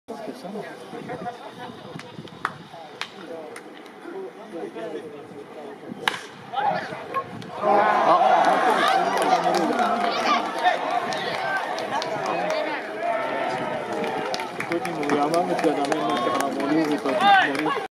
桒原樹 ツーベースヒット 20180515 広島カープ 二軍
桒原樹
ナゴヤ球場 ウエスタンリーグ 中日ドラゴンズvs広島東洋カープ戦にて